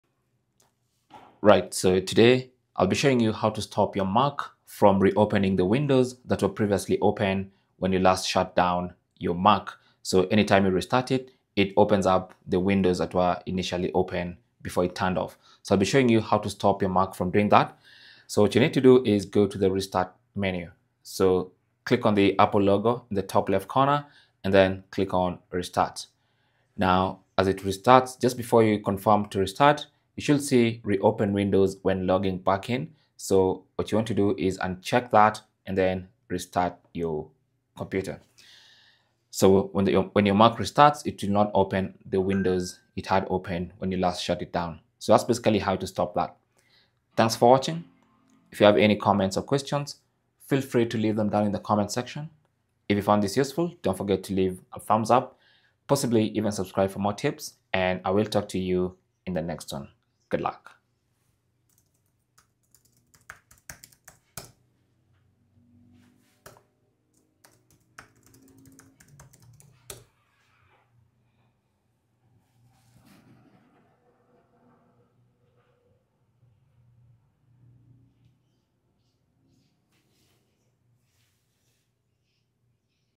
How To Stop Mac From Opening Previous Windows During Start Up
Tired of your Mac reopening all your previous windows every time you start up?  Learn how to stop this feature and enjoy a clean slate when you power up your Mac! 💻✨ In this easy step-by-step tutorial, I’ll show you how to tweak your settings for a smoother startup experience. Perfect for beginners and experienced users alike.

Tap on the Apple logo at the top right corner
Click on restart
Before you select restart,
Uncheck reopen windows when login back in

Cell Phone Tripod Adapter